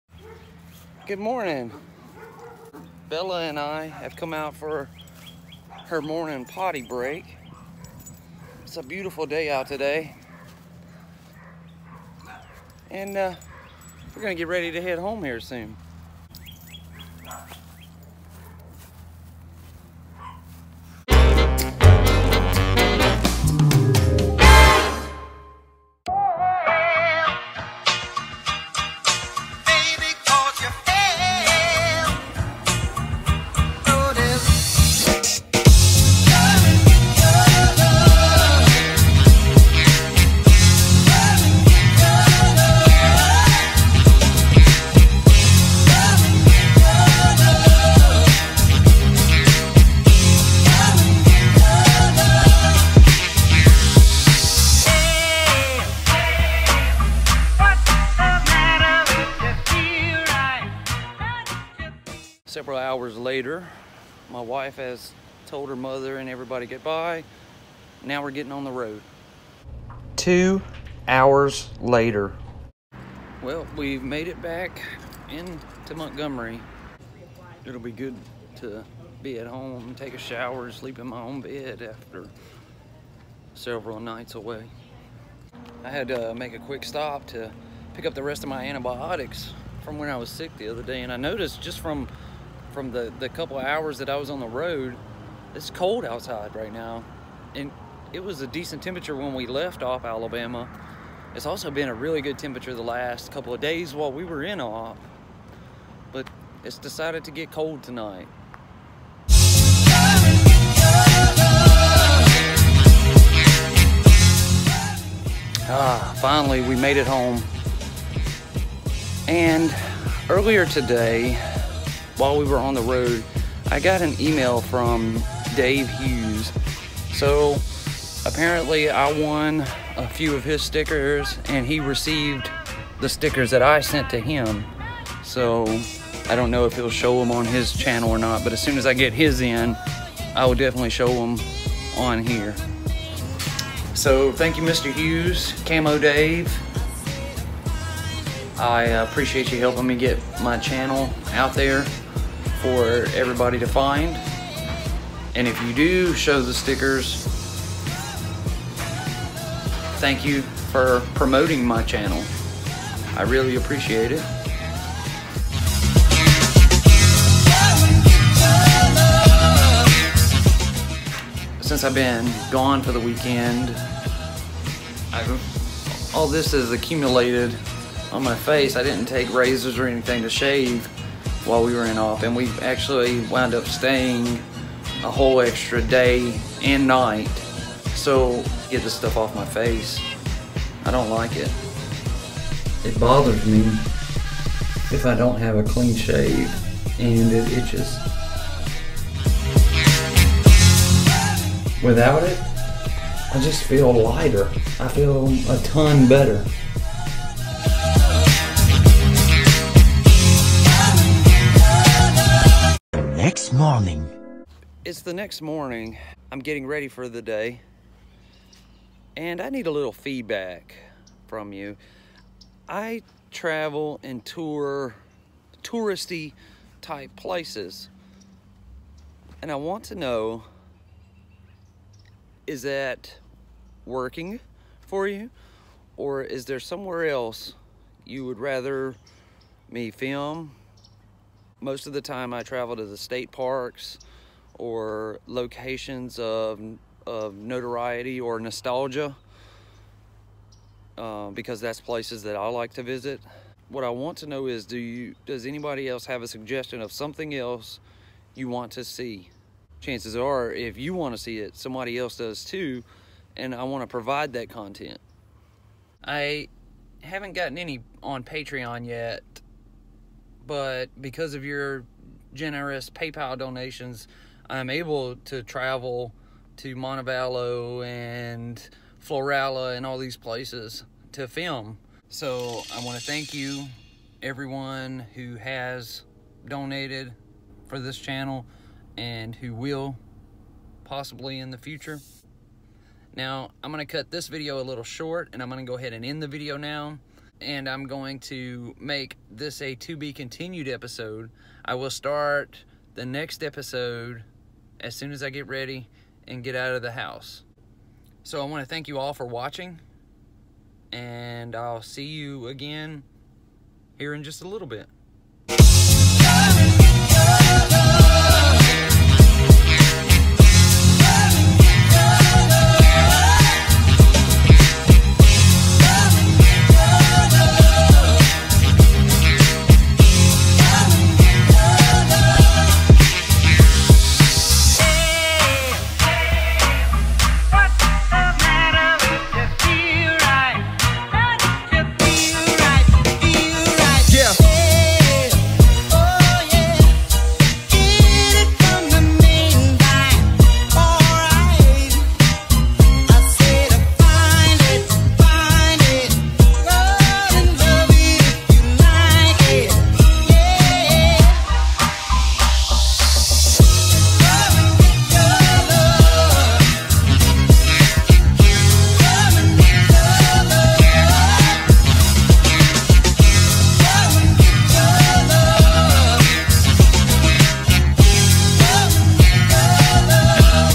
Dave Hughes Stickers
Join me for episode 88 is we travel home from Opp, Alabama and it’s pretty boring other than that thank you all for watching, leave me a thumbs up and subscribe. Part Two will be available in just a few hours

Michael R Williams
1500 Baffin Court
Montgomery, Alabama
36110